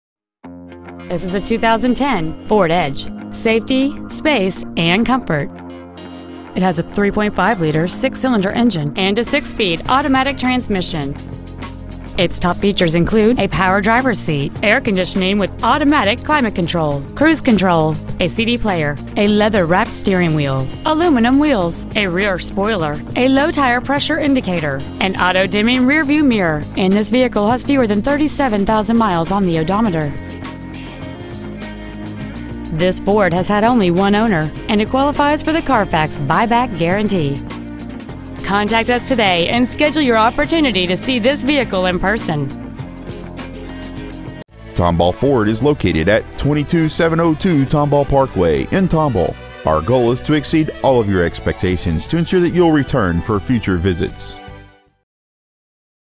2010 Ford Edge Tomball TX 77375
Year : 2010

 Make : Ford

 Model : Edge

 Engine : Gas V6 3.5L/213

 Trans . : Automatic

 Exterior : Ingot Silver Metallic

 Miles : 36,313

 Interior : Charcoal Black

 Stock : ABB59661

 Tomball Ford

 22702 Tomball Parkway